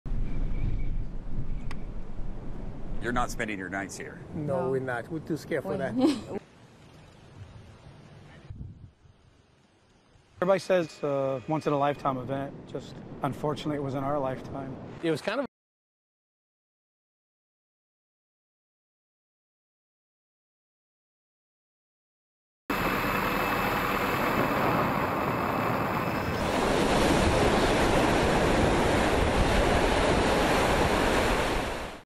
الحمم البركانية تجبر سكان هاواي على مغادرة منازلهم
الحمم البركانية تجبر سكان بيغ آيلاند على مغادرة منازلهم…